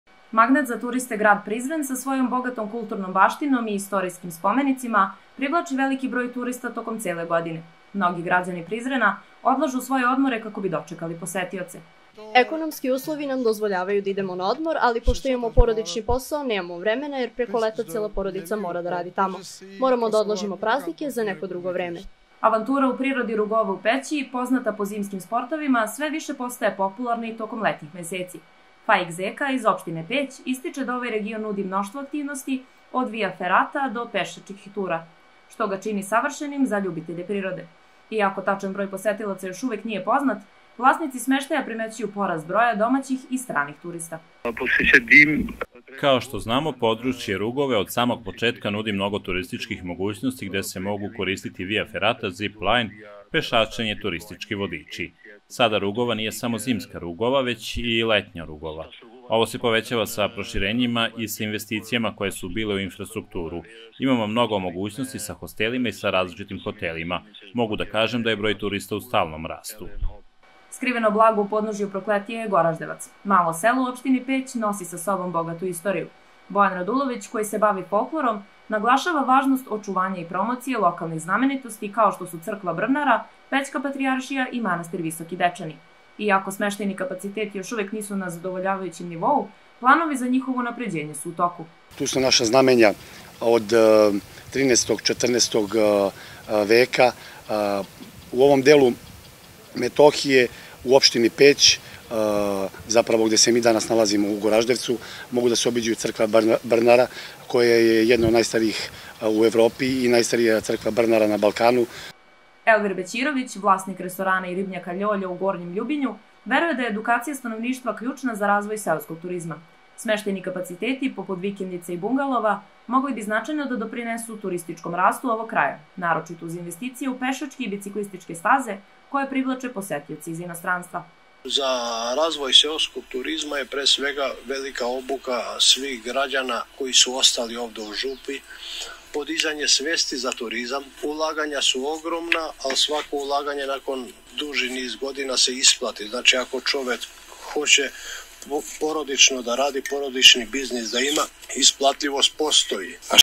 Skriveni dragulji opština Peć i Prizren
Kosovo, sa svojim raznolikim pejzažima i bogatom istorijom, krije mnoge turističke dragulje koji tek treba da budu potpuno otkriveni. Gradovi kao što su Peć i Prizren, zajedno sa manjim naseljima poput Goraždevca i Gornjeg Ljubinja, nude izuzetne mogućnosti za turiste, ali još uvek ne koriste svoj potencijal u potpunosti.